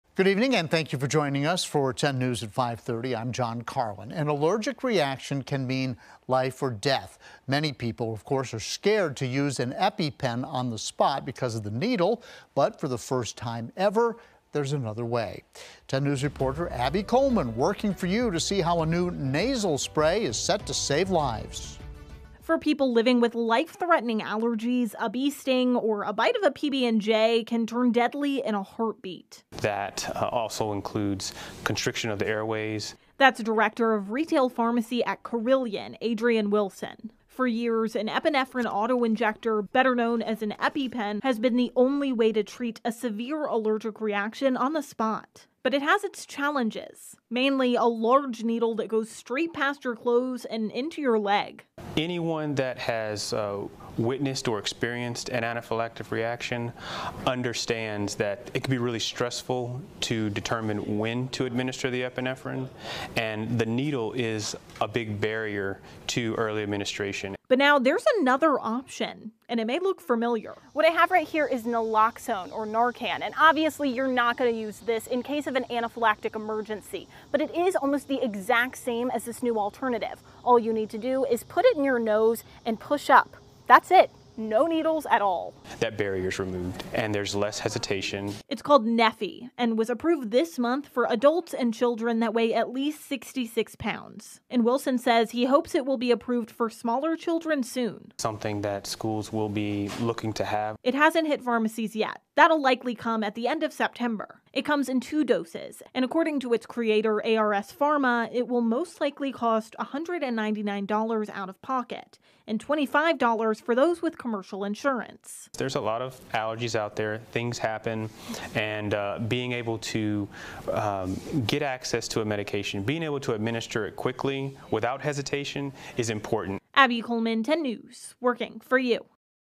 New nasal spray saving lives during allergic reactions
For the first time ever, there may be a new alternative to the EpiPen shot.